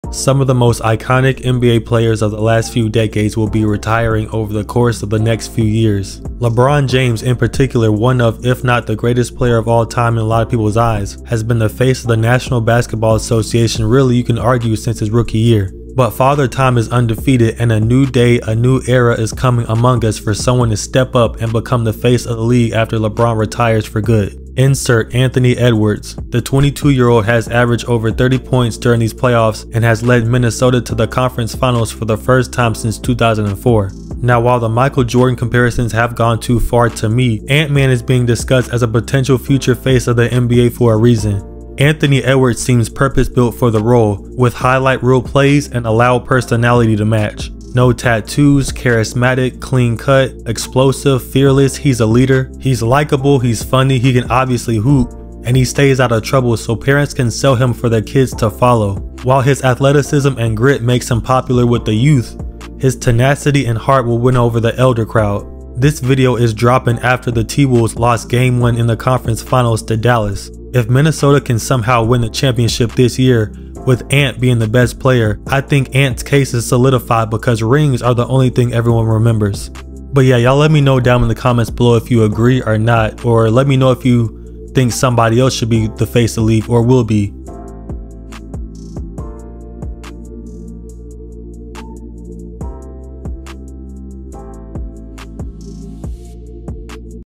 Meet the Potential Face of the NBA
The next face of the NBA?

Anthony Edwards and the Minnesota Timberowlves have been the story of the NBA Playoffs so far. After registering their first series sweep in franchise history when they dispatched the Phoenix Suns in the first round, the Timberwolves have shockingly taken the first two games of their series with the defending champion Denver Nuggets. In the process, Anthony Edwards has emerged as the breakthrough star of the playoffs and now has a golden opportunity to establish himself as the next face of the NBA…